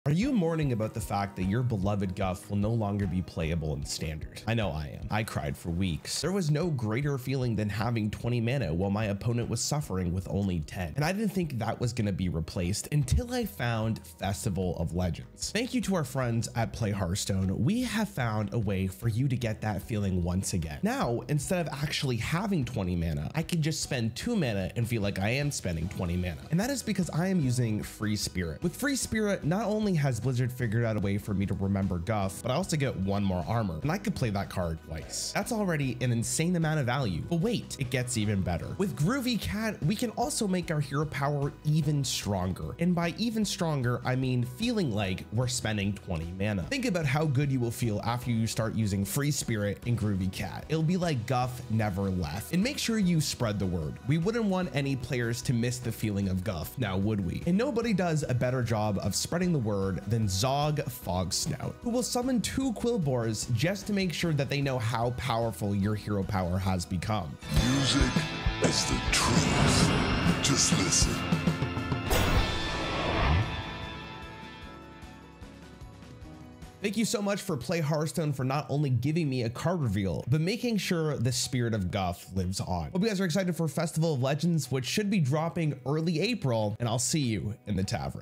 My Card Reveal For Festival of Legends
Rarran reveals a brand new druid card for the new Hearthstone expansion, Festival of Legends